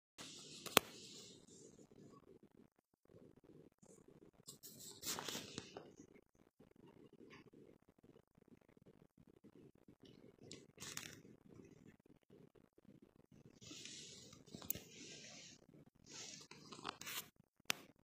A special bond between two special kitties
Prepare yourself. Your heart is about to melt. Meet Bonx and Taco. Bonx is our derpy derp, special kitty with short-length black fur who likes to sleep on his face. Taco is the orange tabby kitten who walks like a drunken sailor sometimes. Two semi-broken kitties complete one another. Their love and support of each other is heartwarming. Today, Bonx just needed a soft, warm pillow. They are up for adoption together.